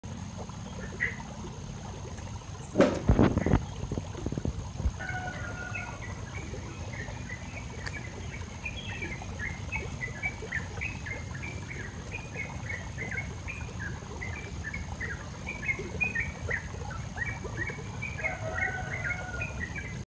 eating Water spinach
hello everyone!
let's see, they are my  turtle and tortoise...
hopefully it is fun and entertaining#turtle